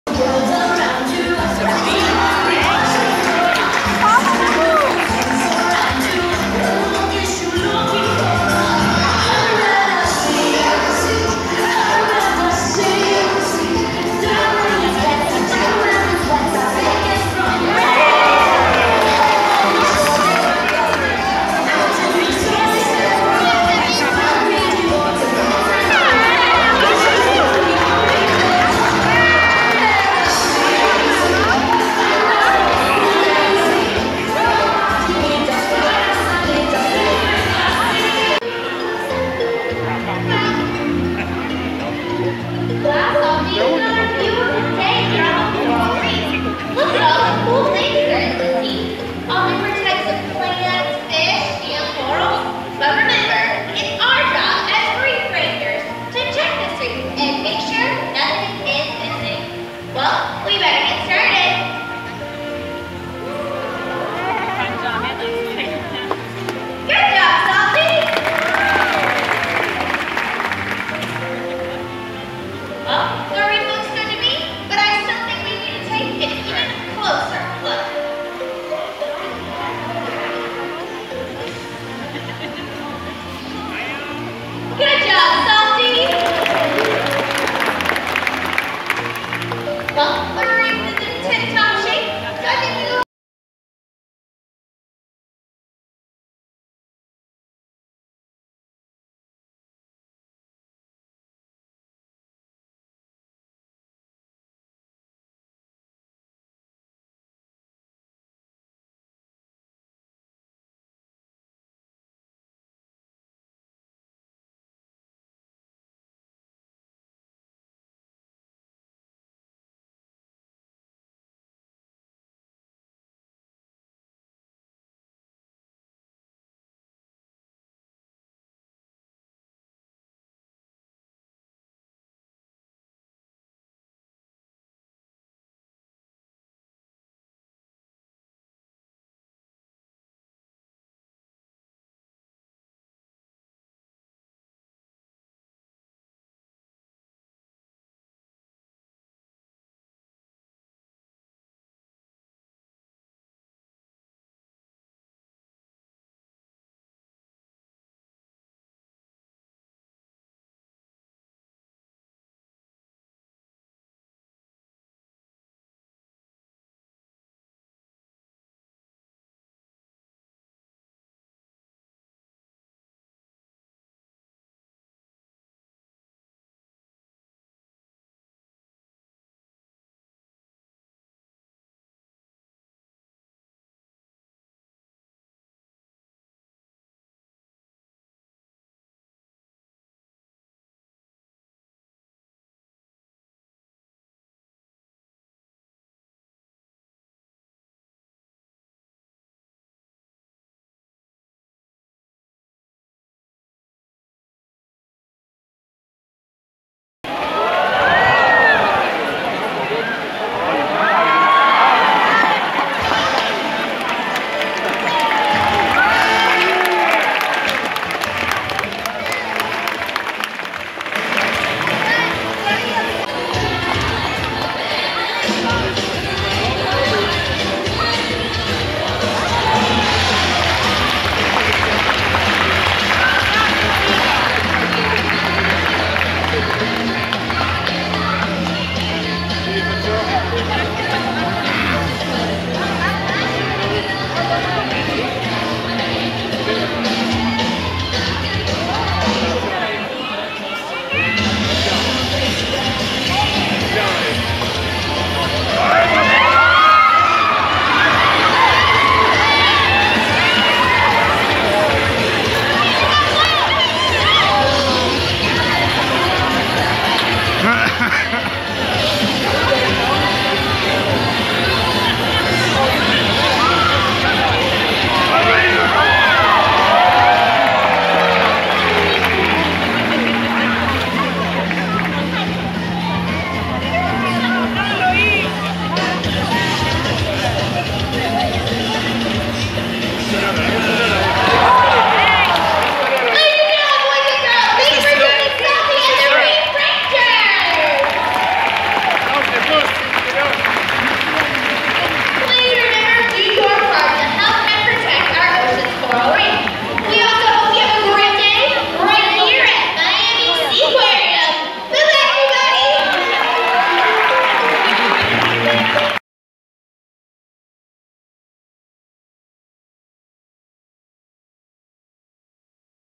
Best Sea Lion Show at Miami Seaquarium | Funny Smart Cute Sea Lion Show
Throughout the day, guests may enjoy the Seal and Sea Lion Exhibit, where they can observe Sea Lions and Seals as they frolic in the South Florida sun at the Miami Seaquarium...